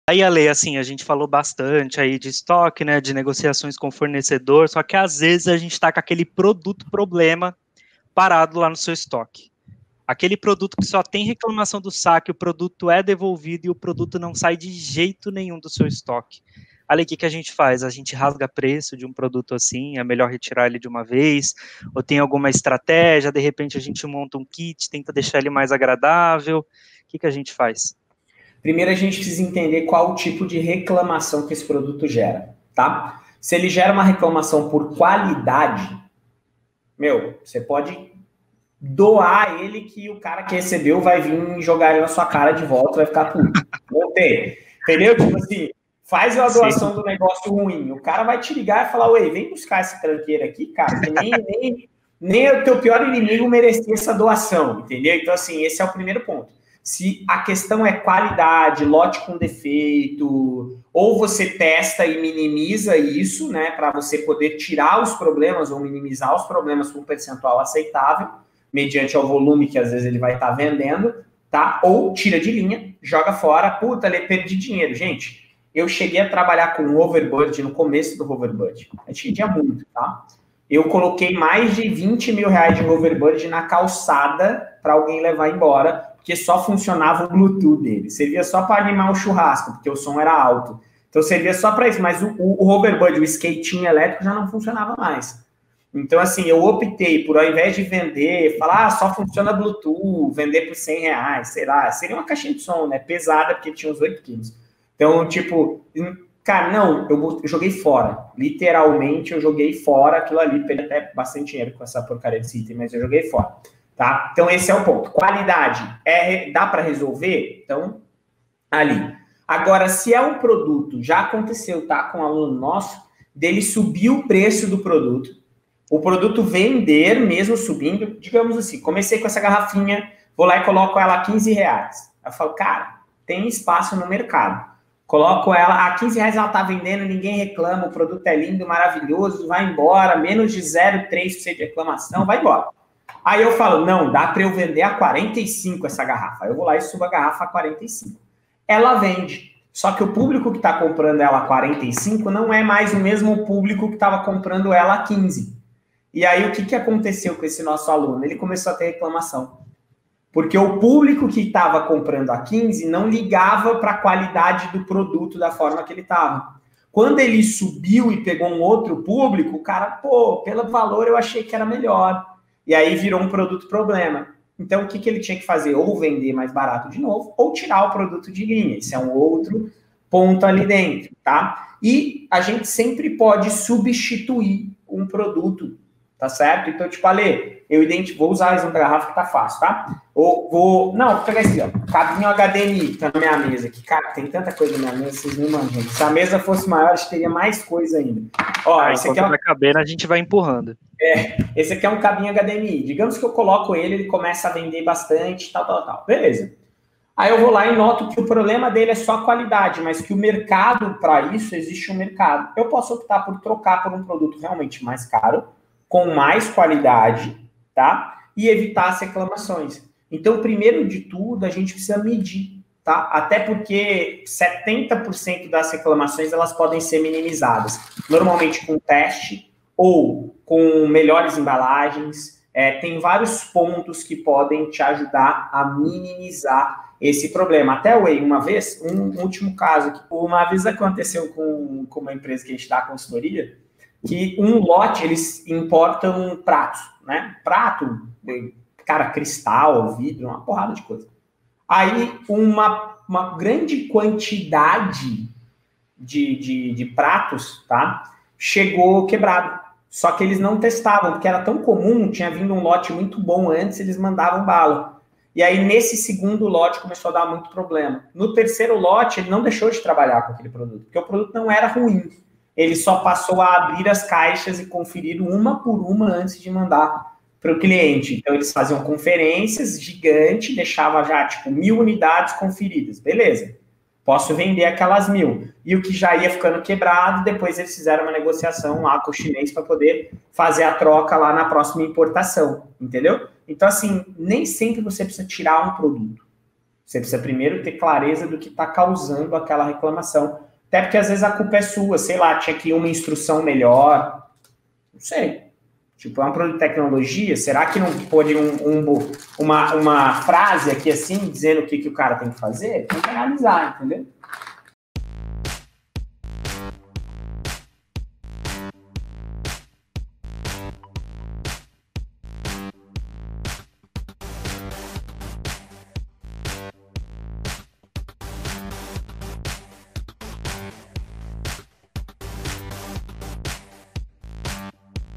O que fazer com um Produto Problema parado em estoque? B2W Americanas Shoptime Submarino
Acompanhe 4 Consultorias ao vivo para criar um plano de ação e alcançar 4 MIL TODO SANTO DIA EM 2021 nas suas vendas

O que fazer com aquele produto problema parado no seu estoque, aquele produto que só tem reclamação do SAC, o produto é devolvido e o produto não sai de jeito nenhum do seu estoque. O que a gente faz? Rasga preço de um produto assim, é melhor tirar ele de uma vez ou tem alguma estratégia, de repente montar um kit, tentar deixar ele mais agradável, o que a gente pode fazer?

Primeiro a gente tem que entender qual o tipo de reclamação que esse produto gera, se ele gera uma reclamação por qualidade, você pode até doar ele e a pessoa que recebeu vai vir jogar ela sua cara de volta.

Se a questão é qualidade do lote com defeito, ou você testa e minimiza isso para você poder tirar os problemas ou minimizar os problemas para um percentual aceitável mediante ao volume que às vezes ele vai estar vendendo ou tira de linha e joga fora.

Se é um produto que subiu o preço, o produto vender mesmo subindo, exemplo, uma garrafinha, vou lá e coloco ela R$15 tem espaço no mercado, ela está vendendo, ninguém reclama, o produto é lindo maravilhoso. Aí eu falo, dá para vender a R$45 essa garrafa, só que o público que está comprando ela R$45 não é mais o mesmo público que estava comprando ela R$15, e então o que acontece? Você começa a ter reclamação, porque o público que estava comprando a R$15 não ligava para qualidade do produto, quando o valor dele subiu e pegou um outro público, o cliente fala que pelo valor achou que o produto era melhor, então virou um produto problema. O que tem que fazer? Ou vender mais barato de novo ou tirar ele de linha.

E a gente sempre pode substituir um produto, por exemplo, um cabo HDMI. Digamos que eu coloco ele e começa a vender bastante, então eu vou lá e noto que o problema dele é só a qualidade, mas que o mercado para isso existe, eu posso optar por trocar para um produto mais caro e com mais qualidade e evitar as reclamações.

Então o primeiro de tudo a gente precisa medir. Até porque 70% das reclamações podem ser minimizadas com um teste ou com melhores embalagens, tem vários pontos que podem te ajudar a minimizar esse problema.

Nem sempre você precisa tirar um produto, você precisa ter clareza do que está causando aquela reclamação.

Apoiam nosso projeto:
Cupom: umarketplace

Cupom: PARCEIRO20